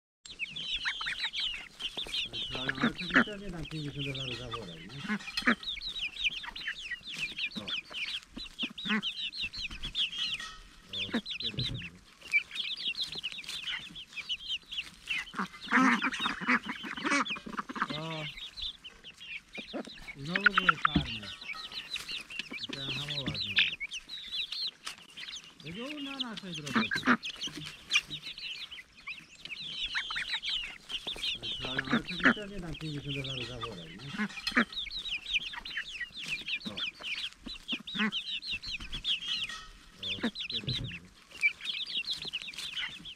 This pot of rice is mine now - Young hens feed as one take center stage
Young hens feeding on cooked rice with some corn as one of chicken step right into pot taking center stage as animals being held in temporary small outdoor enclosure while new large enclosure is under construction.

To download full resolution and obtain license from Pond5 Realty-free Videos